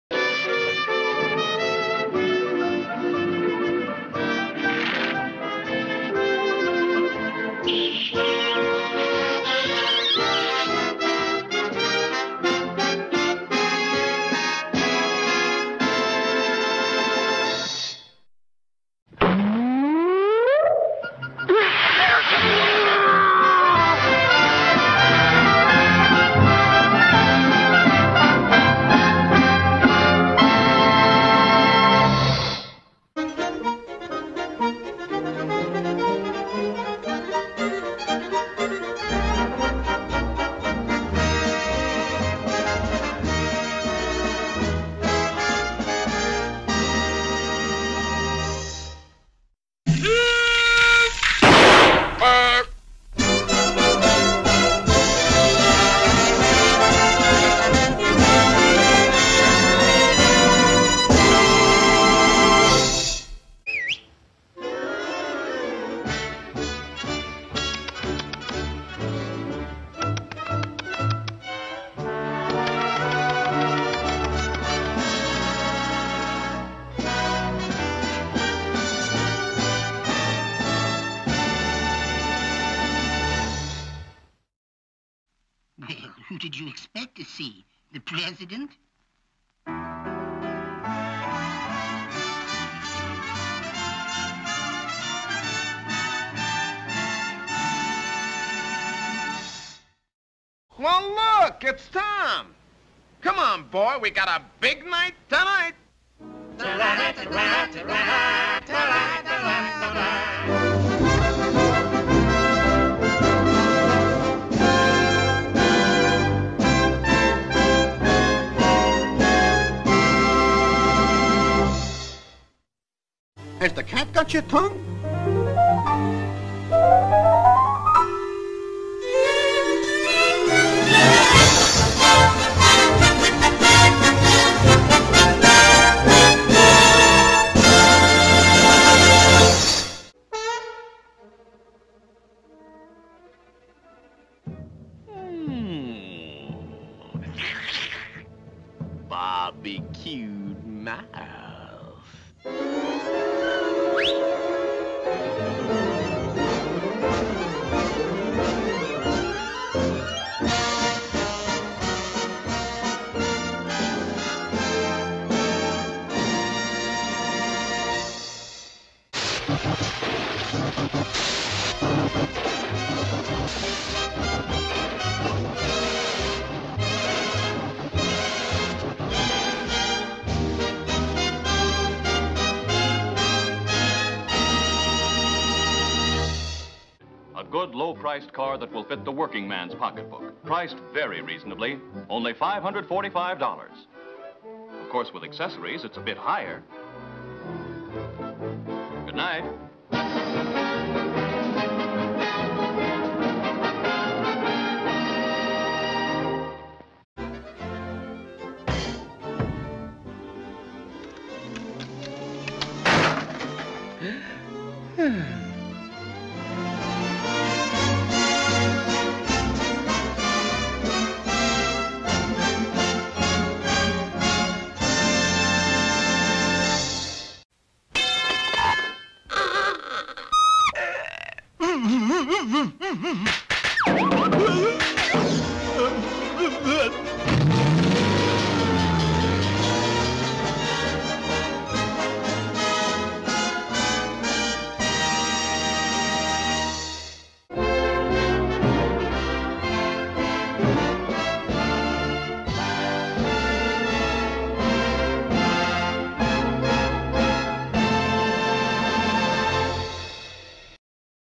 Every MGM Cartoon Studio End Titles (1950-1952) Part 2 **re-upload**
Released from January 6, 1951 to December 8, 1951.

The final MGM cartoon in a reissue before the studio shut down in August of 1958.
Directed by Various MGM Cartoon Directors.
Produced by Fred Quimby.